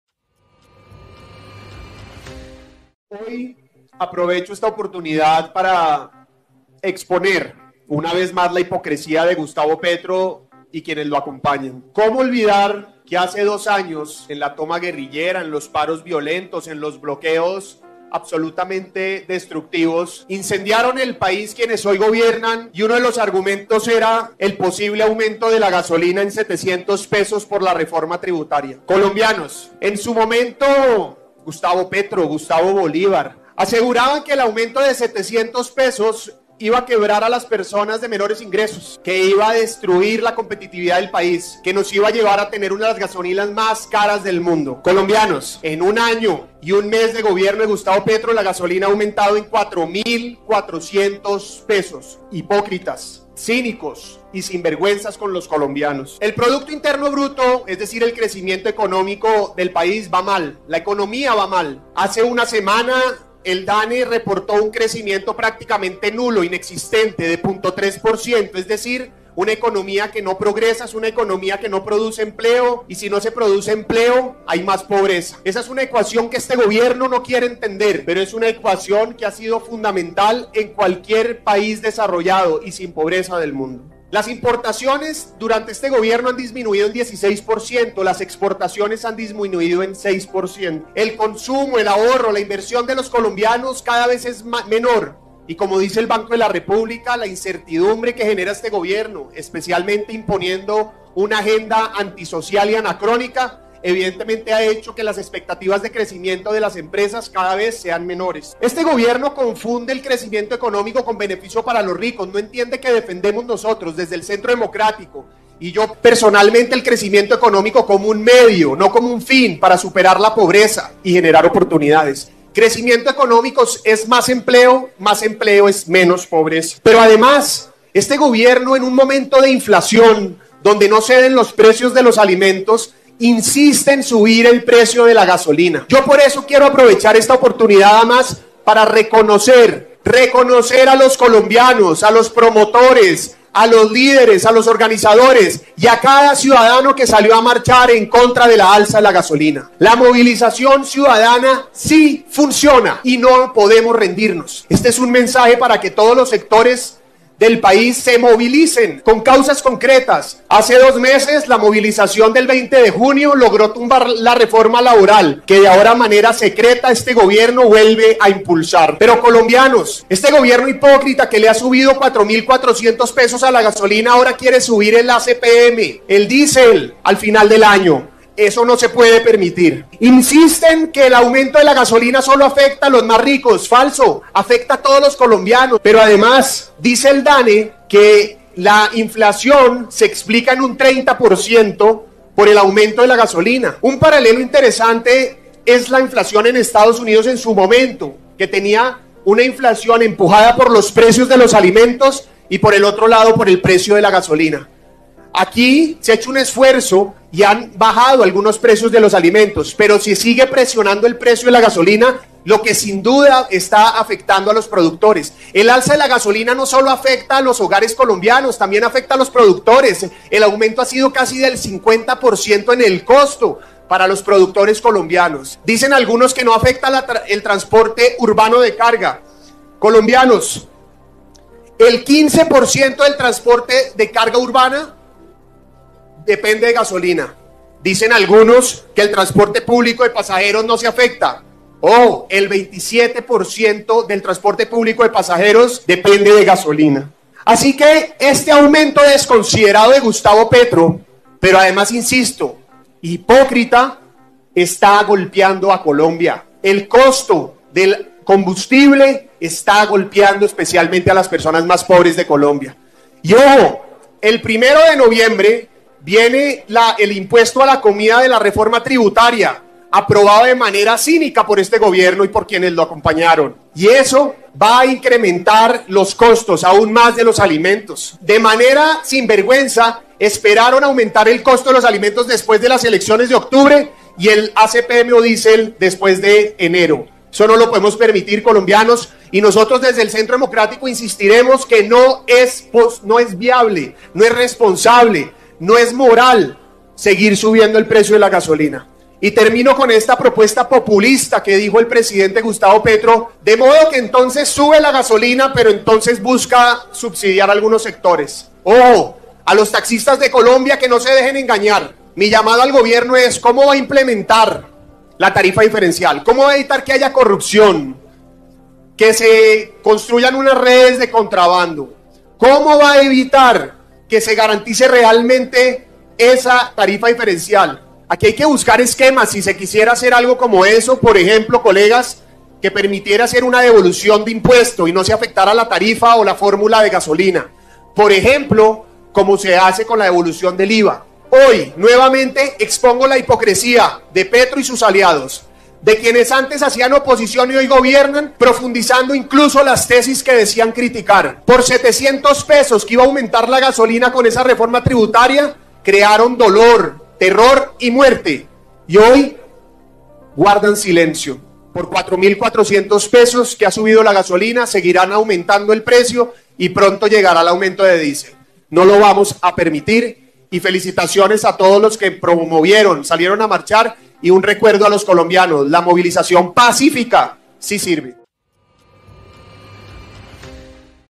¡GOBIERNO HIPÓCRITA! 🔴 URGENTE 🔴 MIGUEL URIBE A PETRO SOBRE EL ALZA A LA GASOLINA
A Petro sobre la alza de la gasolina:
¡Gobierno hipócrita!
MIGUEL URIBE TURBAY